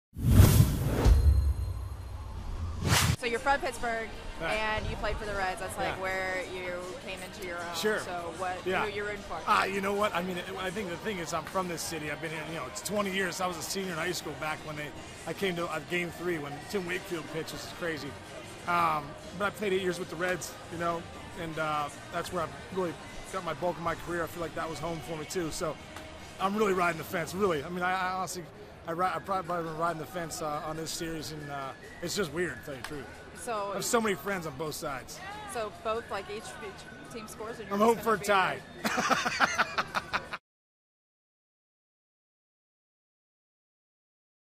Casey reveals rooting interest in NL Wild Card Game
Former Major Leaguer Sean Casey grew up in Pittsburgh and played for the Reds, but who will he root for in the NL Wild Card Game?